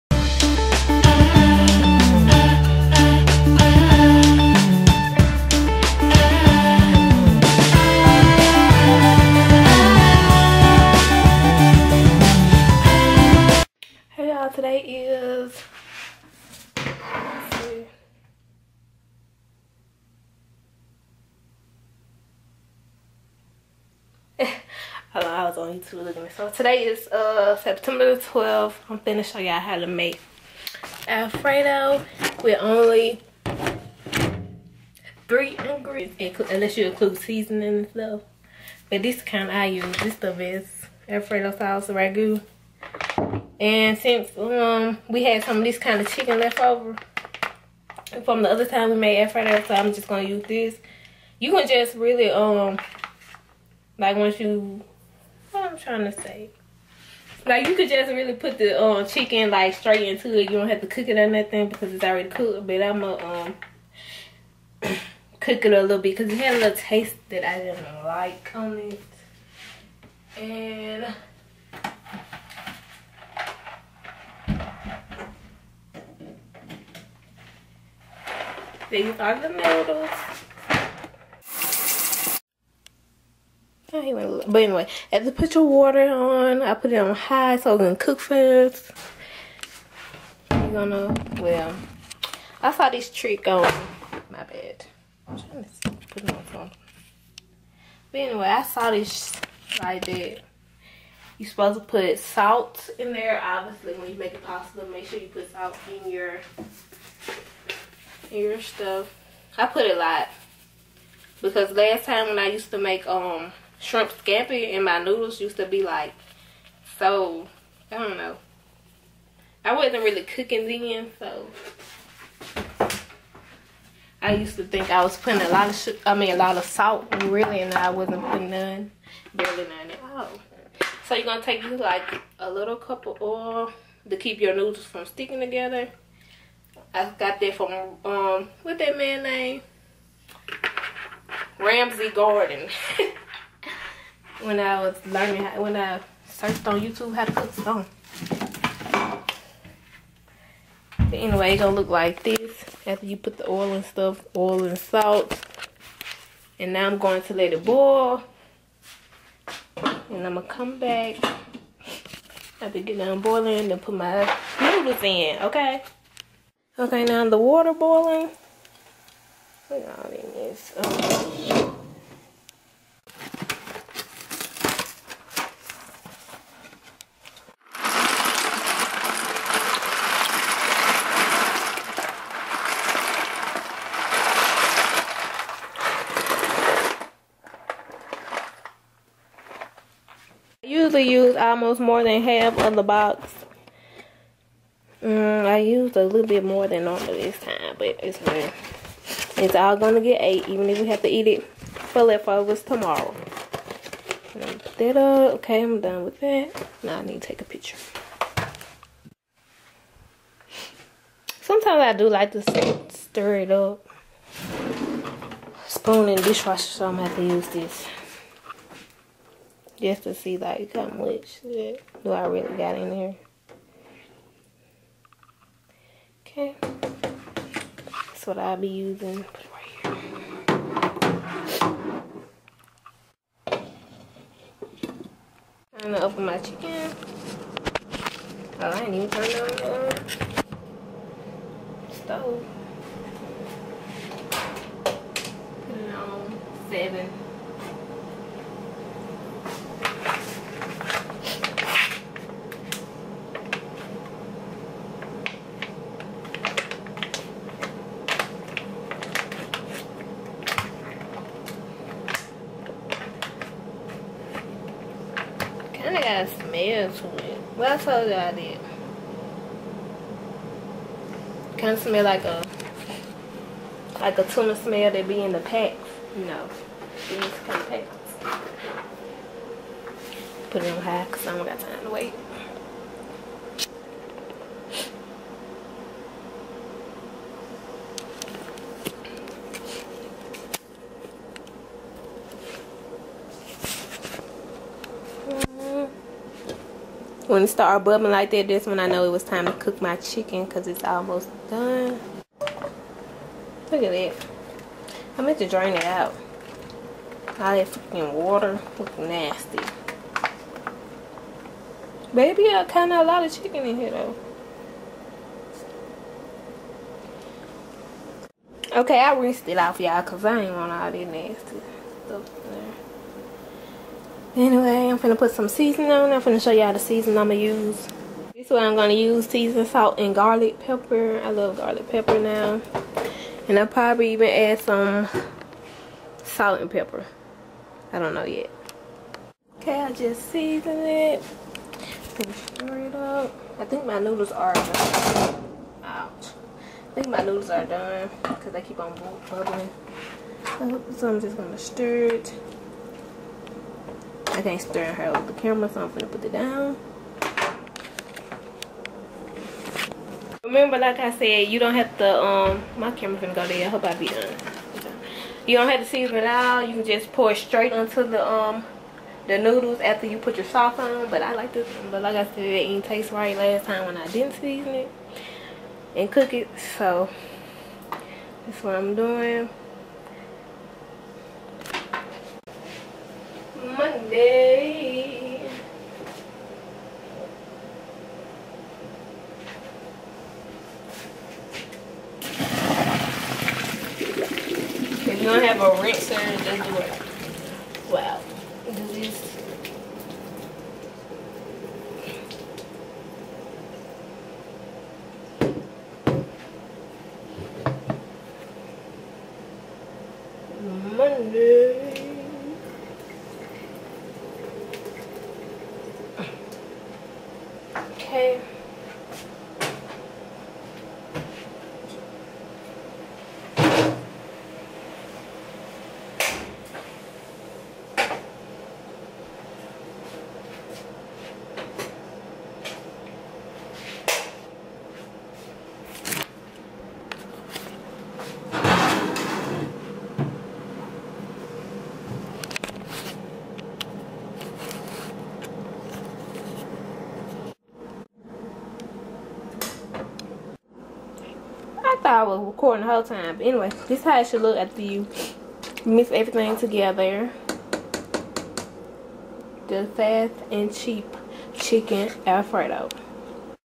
HOW TO MAKE CHICKEN ALFREDO UNDER $5!!
This is a vlog style tutorial on howto make chickenalfredo!
If you follow these steps your chicken alfredo will be delicious!